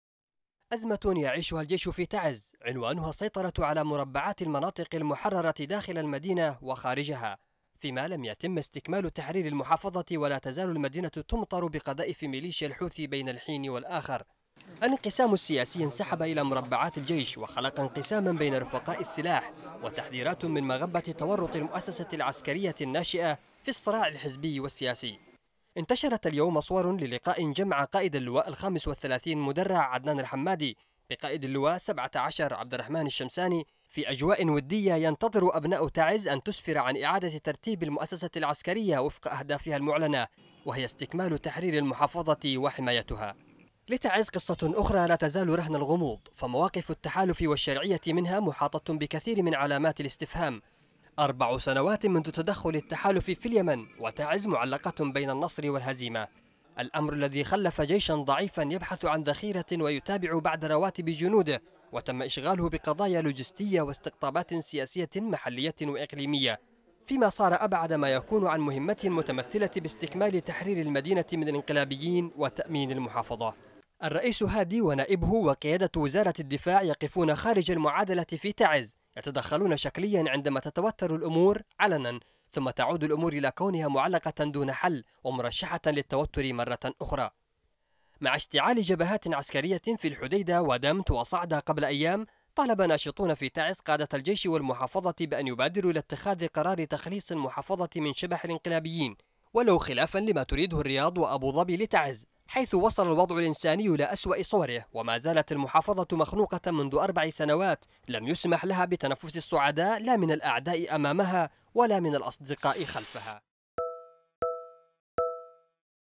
لماذا لايتجه الجيش في تعز الى الجبهات ضد الحوثيين؟ | تقرير : أسامة عادل
من المسؤول عن حالة الانقسام التي يشهدها الجيش في تعز؟
ولماذا لا يتحرك الجيش لجبهات القتال مع الانقلابيين بدل التناحر داخلَ المدن والارياف؟
هل سيكبر قادة تعز على الاستقاطابات الضيقة، ويكونوا بحجم معاناة هذه المدينة وتضحياتها؟